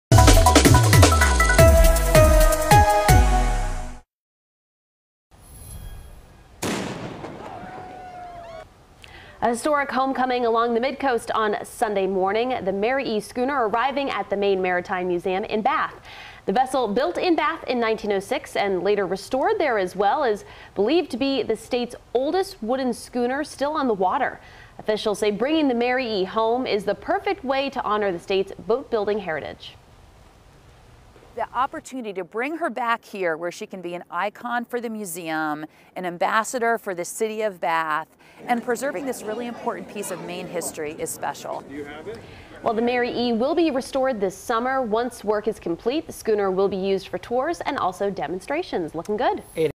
Historic Maine schooner returns hom
The Mary E, built in Bath in 1906, is back to join the Maine Maritime Museum.